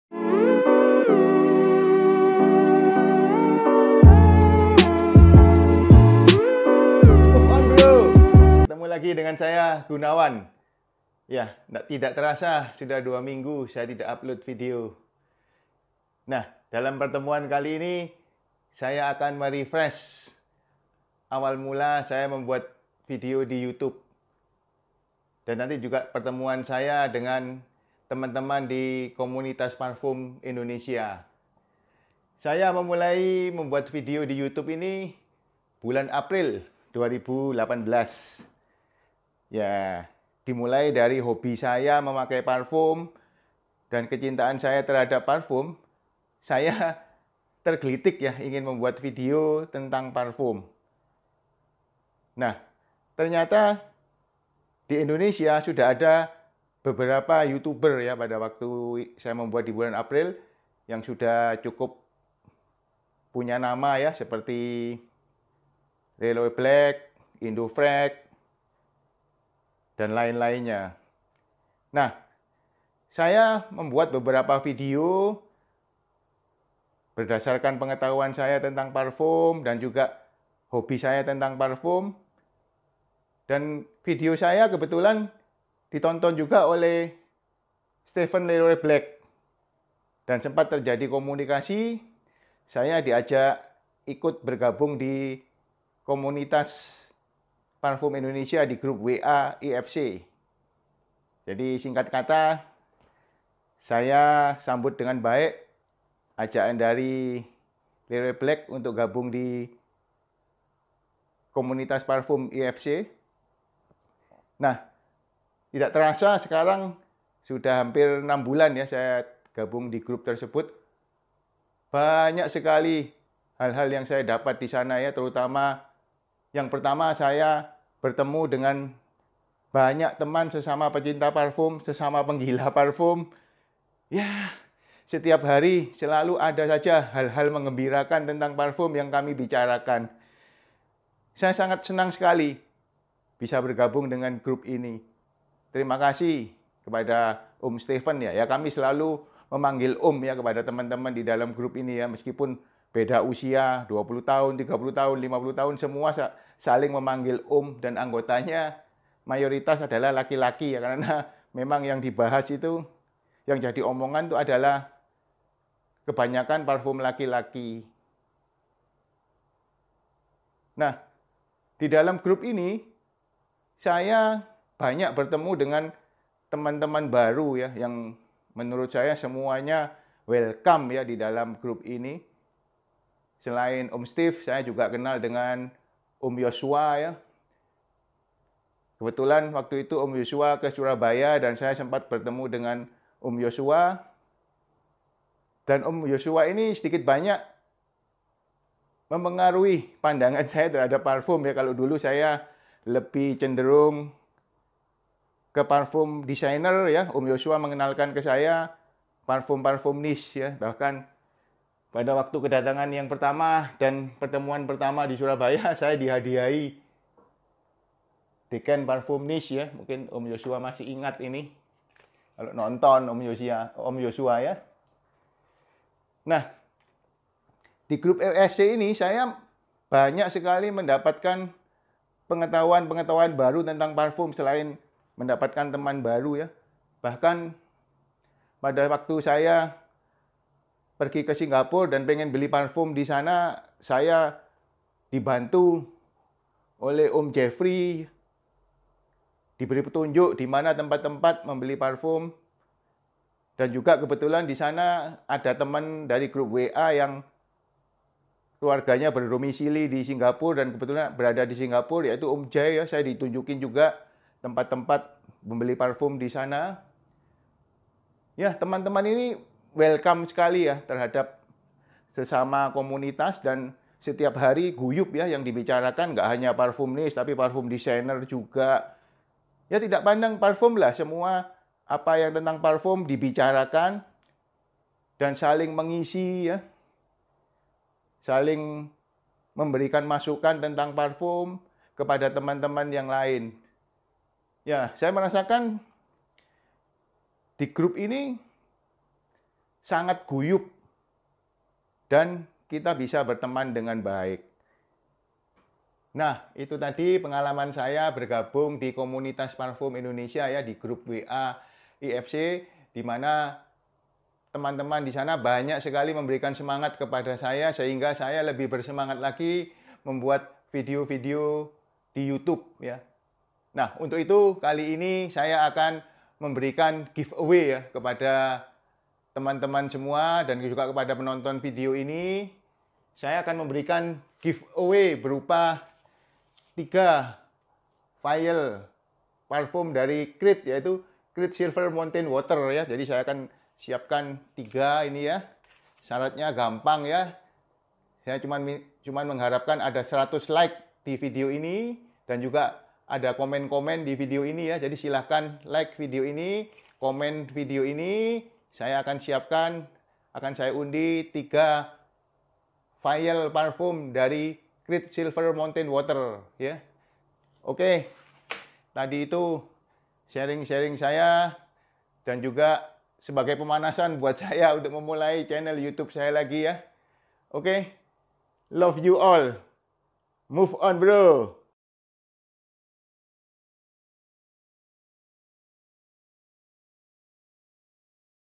Bahagia Komunitas Parfum Indonesia - Parfum Review Indonesia
Refresh sejenak,  awal mula saya membuat video youtube dan perkenalan saya dengan komunitas parfum Indonesia.
Terima kasih sahabat!

Saya berikan juga giveaway Vial Creed silver mountain water 3 pcs.
syaratnya cukup komen dan like.
100 like akan saya undi 3 pemenang.